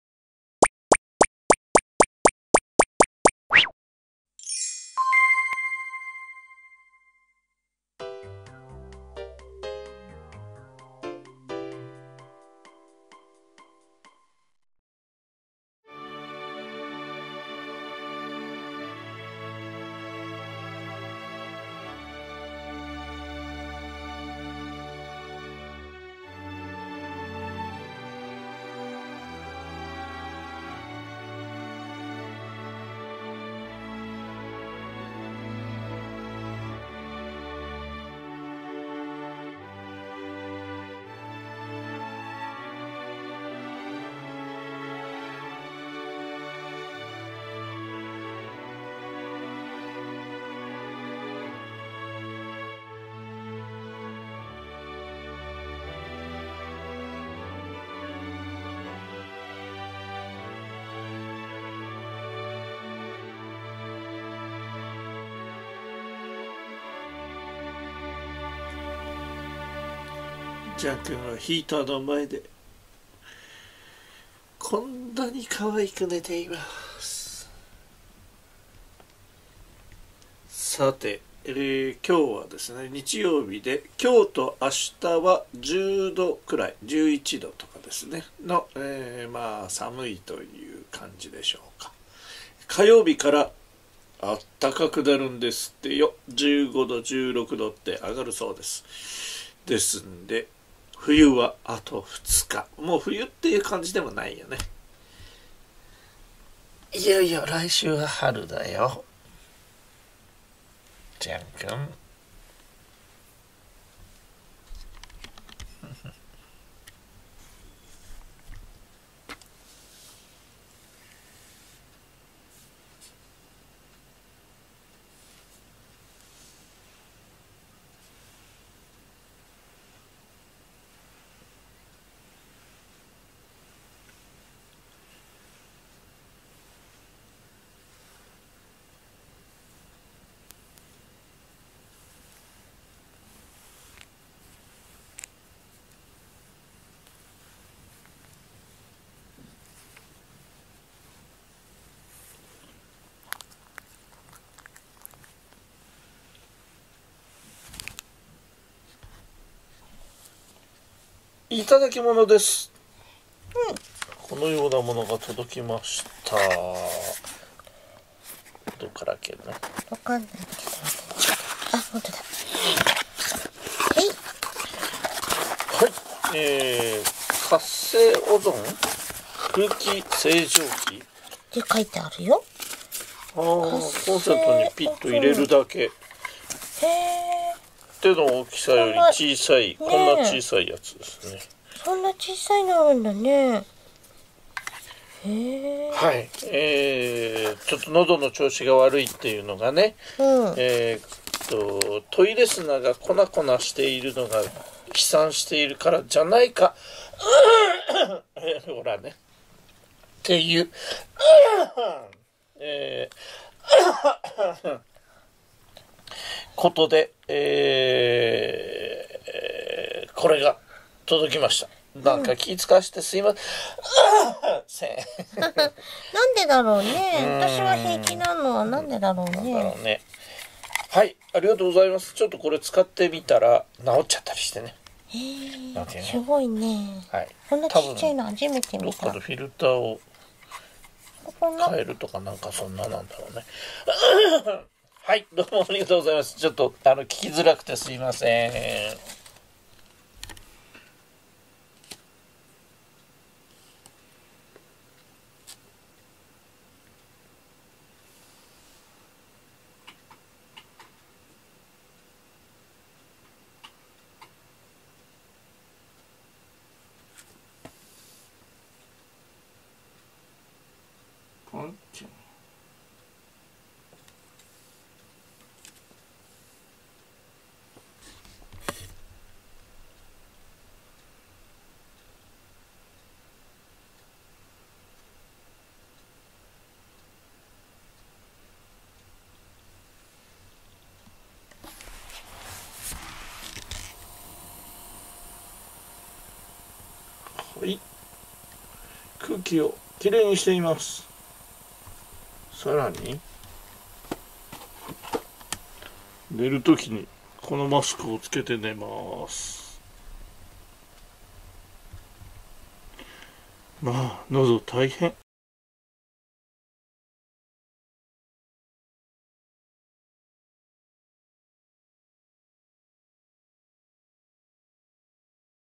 かわいいジャン＆ポンが撮れました！アップ多めです【Jean & Pont 2417】2021/3/7 保護猫ジャンけんポン
ヒーターの前でゴロゴロするジャンくんが可愛くてアップを沢山撮りました。
ポンちゃんもこれでもかというほどのドアップを撮影しましたのでご堪能ください。
くろけんパパの喉の調子が悪いので、超小型のオゾン空気清浄機をいただきました。
ありがとうございます、早速電源を入れました。